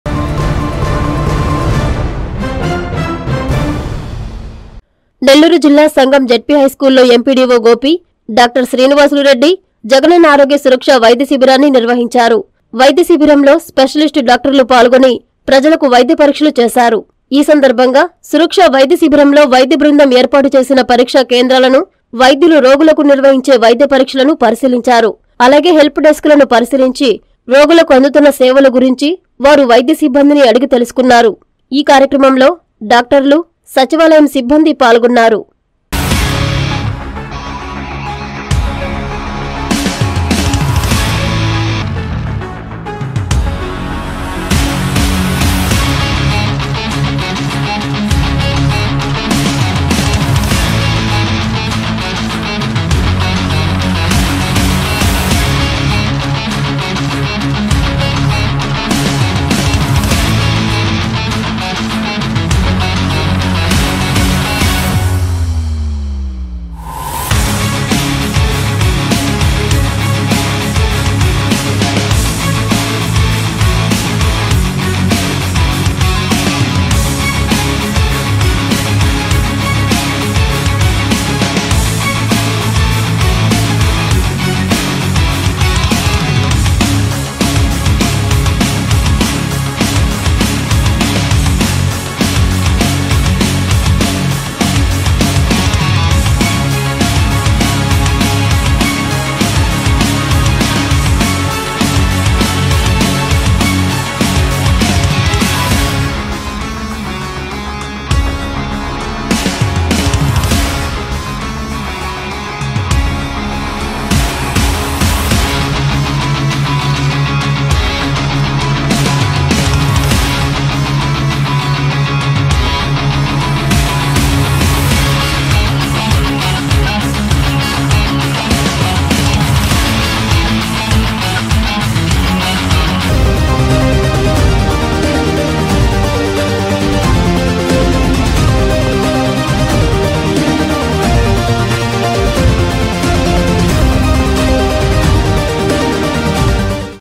సంగంలో జ‌గ‌న‌న్న ఆరోగ్య సుర‌క్ష‌ | Jagananna Arogya Suraksha | Sangam
- వైద్య‌శిబిరాన్ని ప‌రిశీలించిన ఎంపీడీవో గోపి